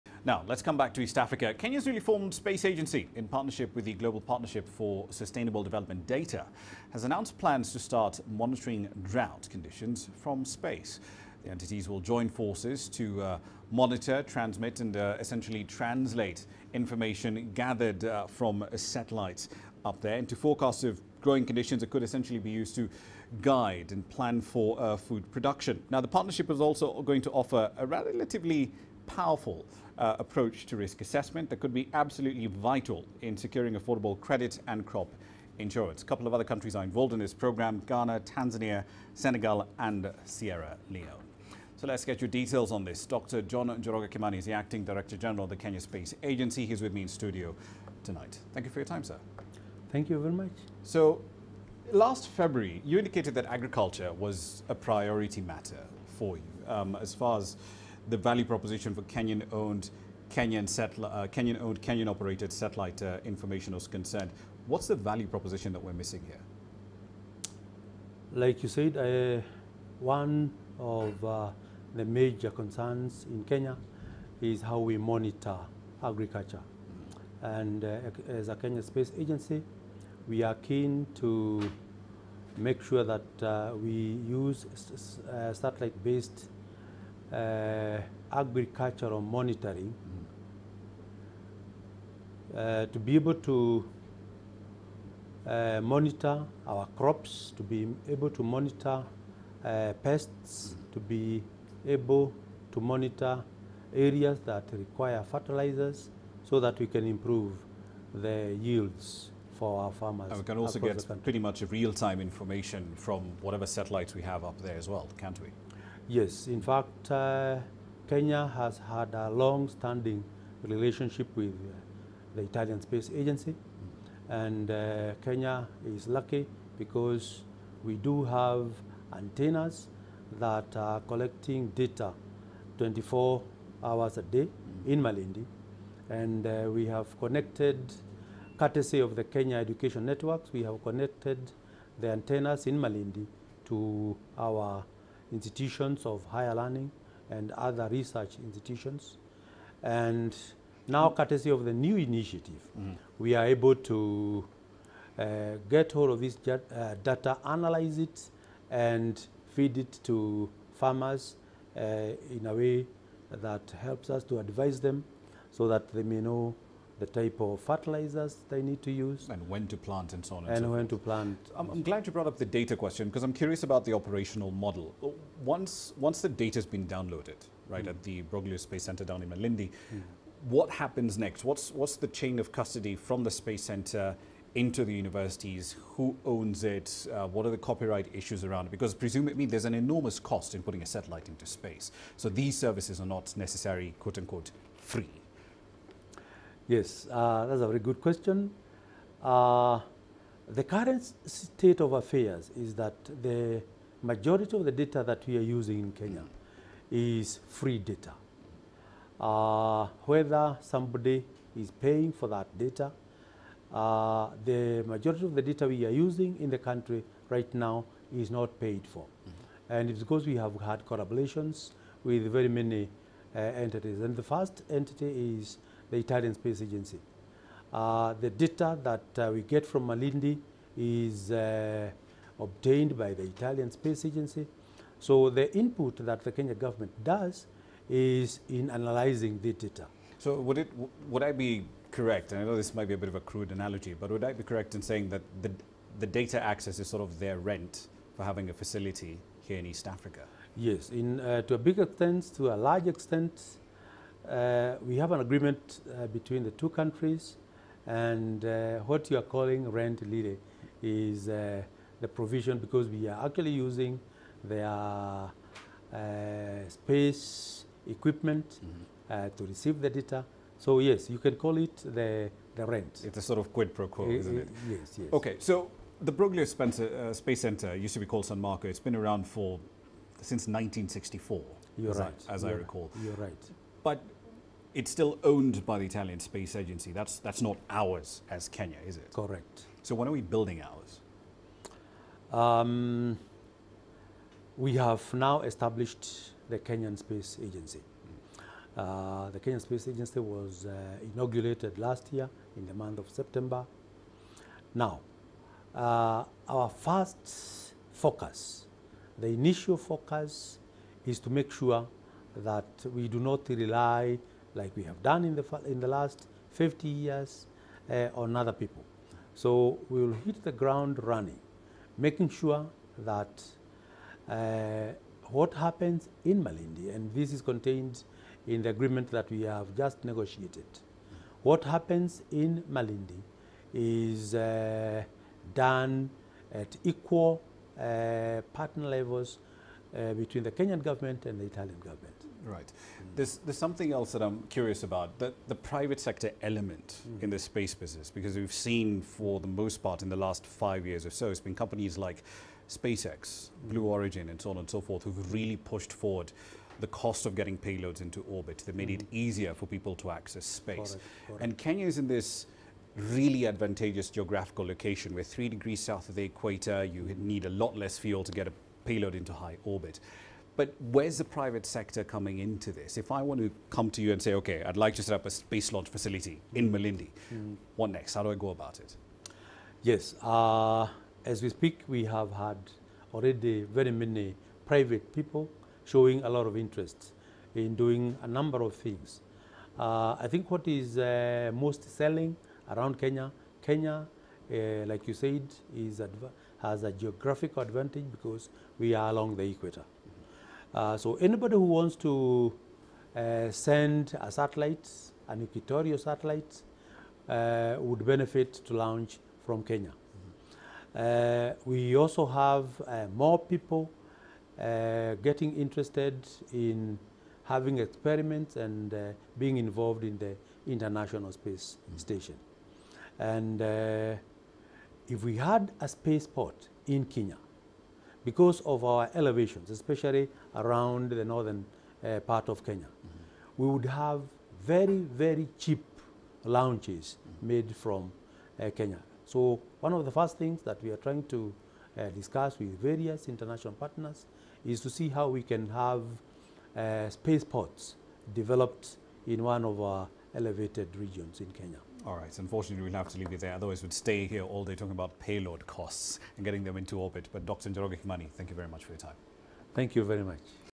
Kenya's Space Agency intensifies use of science to curb drought
The entities will join forces to transmit and translate satellite imagery into forecasts of growing conditions, that could be used to stabilize food production. The partnership will also offer a powerful type of risk assessment, that could be critical in securing affordable credit and crop insurance. Ghana, Tanzania, Senegal, and Sierra Leone are also expected to be part of the Data Cube initiative.